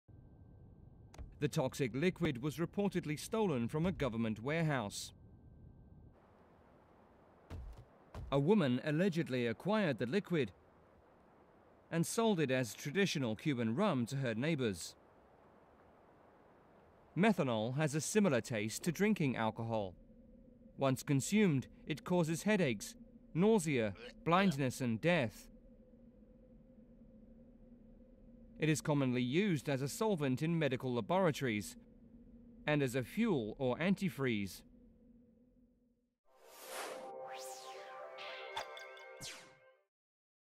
Toxic phony rum kills seven in Cuba, leaving dozens hospitalized
Seven people died and more than 40 people have been hospitalized after they mistakenly drank methanol instead of rum in Cuba.

According to Reuters, the poisoning happened in the La Lisa municipality of Havana, and all those killed were in their 40s and 50s. Local media reports that two individuals who work in the Pharmacy and Food Institute reportedly stole methanol from a local warehouse and passed it to a woman who lives in the same area as those affected. The woman sold methanol as traditional Cuban rum.

Methanol is a light, colorless and flammable liquid with a distinctive odor very similar to drinking alcohol. Once consumed, it causes headaches, nausea, blindness and death. Methanol is commonly used as a solvent in medical laboratories and as a fuel or anti-freeze.